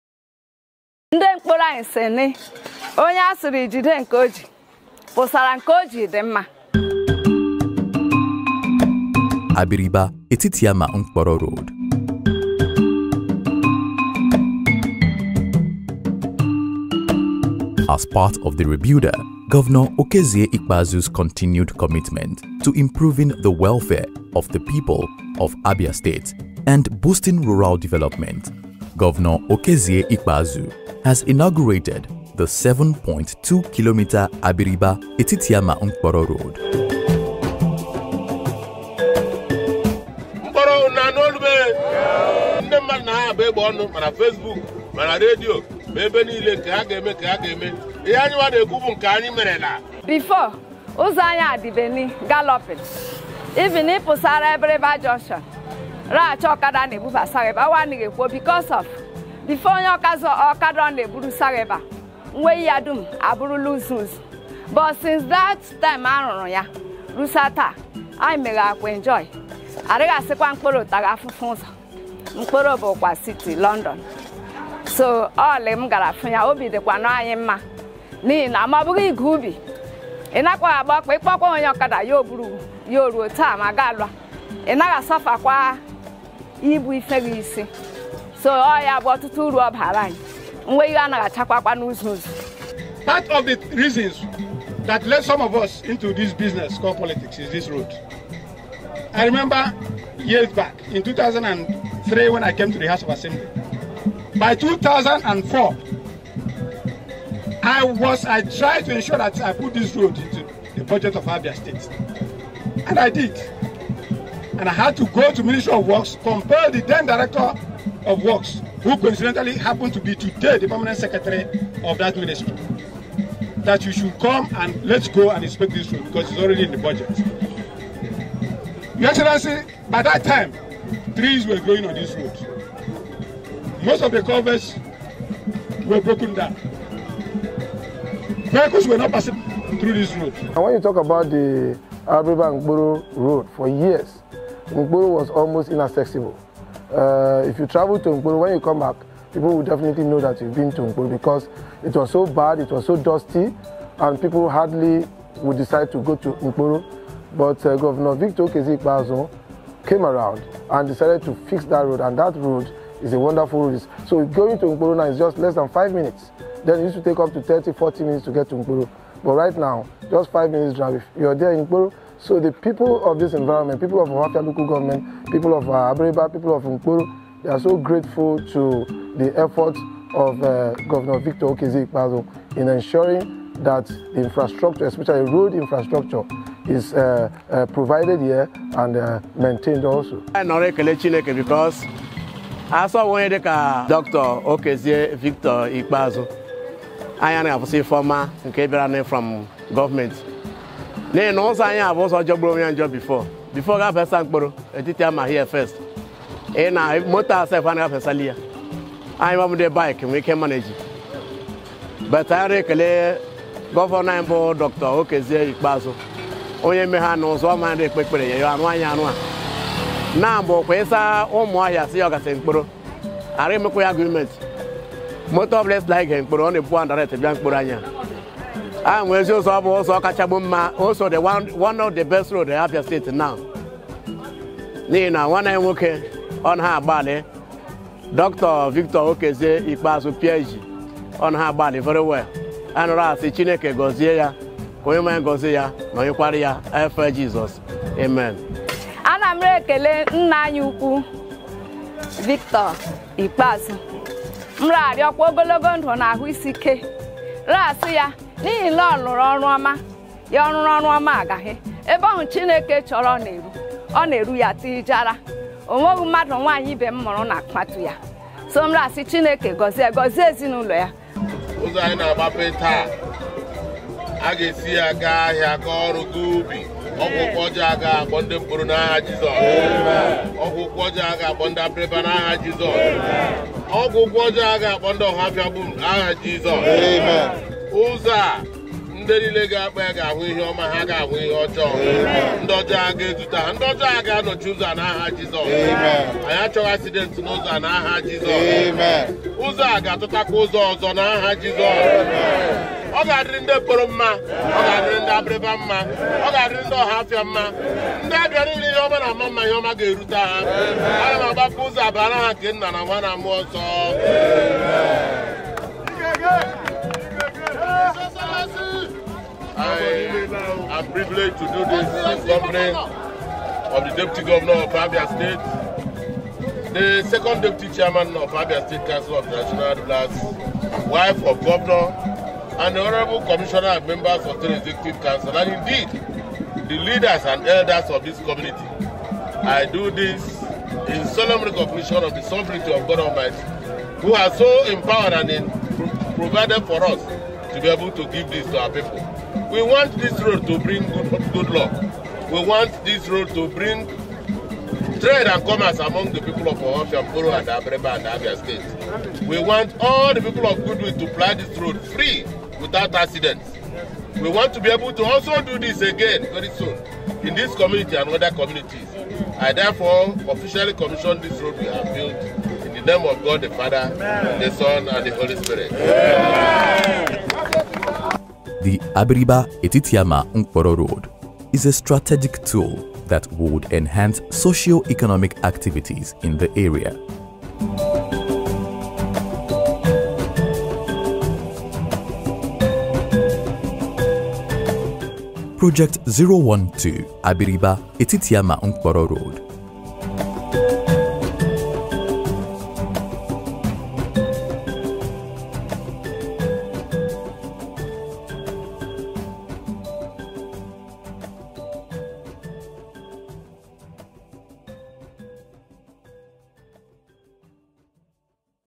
project 012, ABRIBA/ETITIAMA-NKPORO ROAD
NKPORO/ABRIBA ROAD
As part of THE REBUILDER, GOVERNOR Okezie Ikpeazu’s  continued commitment to improving the welfare of the people  of Abia State and boosting rural development,
Governor Okezie Ikpeazu has inaugurated the 7.2km Abiriba/Etitiama-Nkporo road.
the Abiriba/Etitiama-Nkporo road is a strategic tool that would enhance socio-economic activities in the area.
Project 012 the Nkporo/abriba road